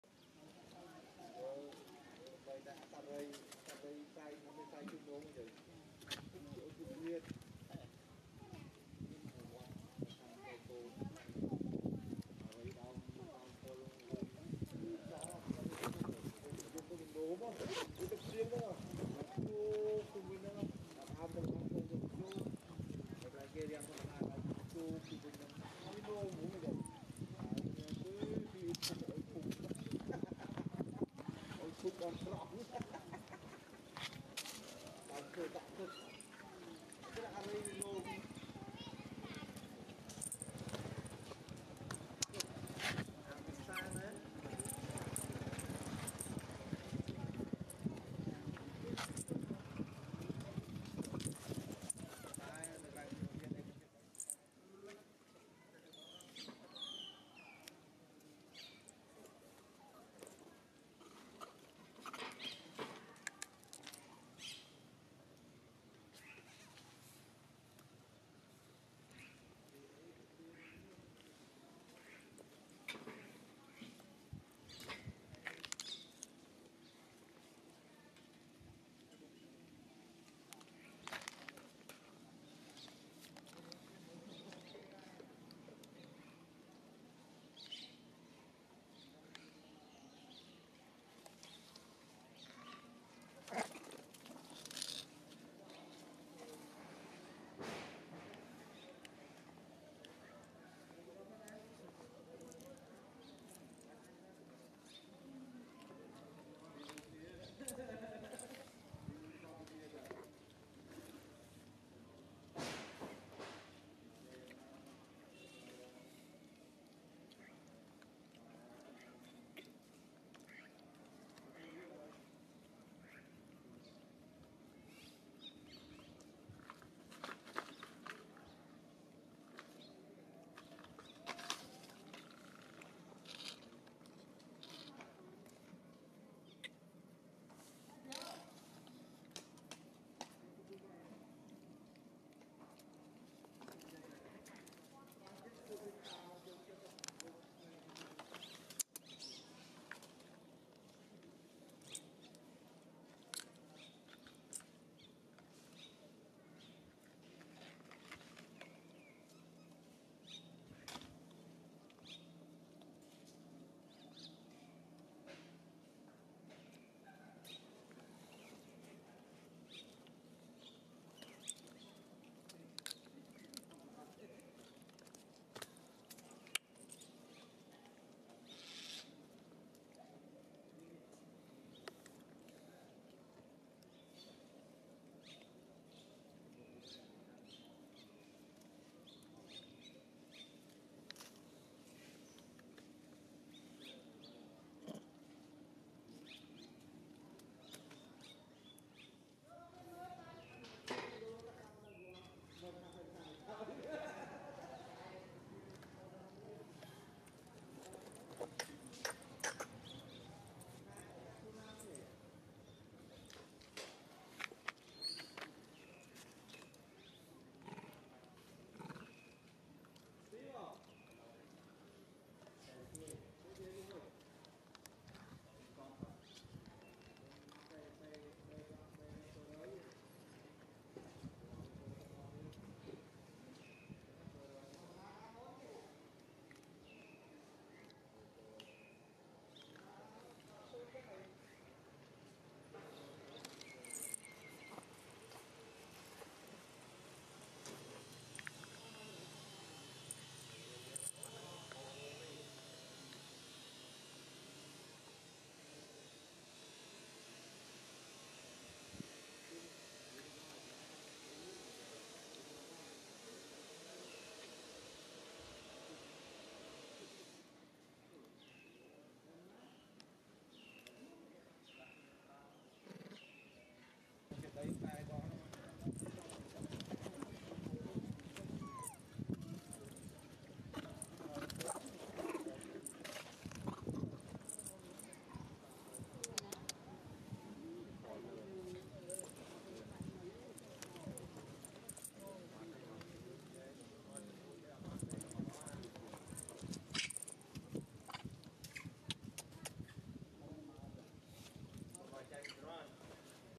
Monkey babies monkey very very happily after gets food from ...
Hello Everyone
This video i want to show you all about:Monkey babies monkey very very happily after gets food from ...
All the video are copyright of My channel ( Animal Room ).
monkey edge ,e monkey survey, emonkey vietnam ,monkey funny, monkey funny face